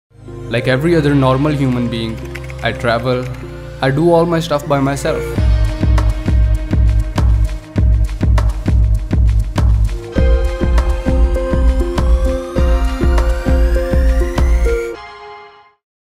(Official) Umar Mughal 15 Second Ad Spot
This is our 15 second teaser trailer for Umar's Feature Story.

Meet Umar, a twenty-eight years old self-dependent man. Umar is a graphic designer by profession and lives in Lahore, Pakistan. He is suffering from a disease known as Osteogenesis imperfecta, in such a disease the bones of a child’s body do not get full strength and they break very easily.

Umar's story is just one of many that we're featuring on Your Everyday Heroes

For more on Umar - follow him on IG @Umar_Mughal10